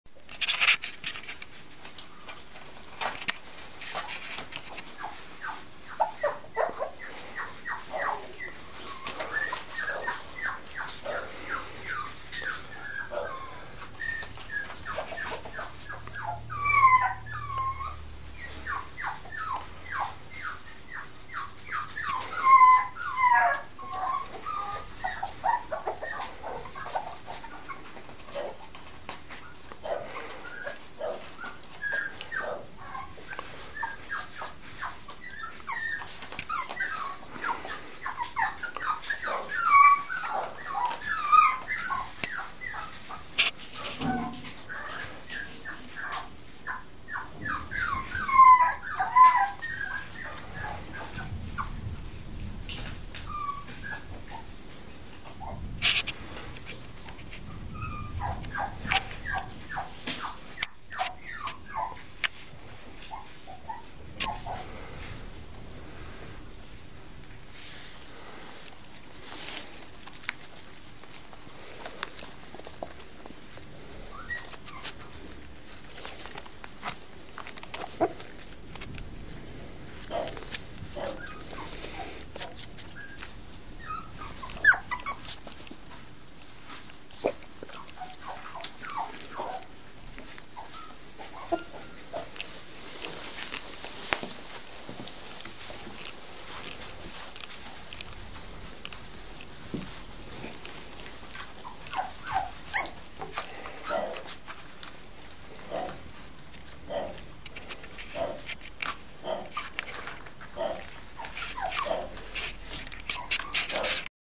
norfolk terrier bitch puppy 8 and half weeks. 05-05-2015
8.5 week old Norfolk terrier bitch puppy.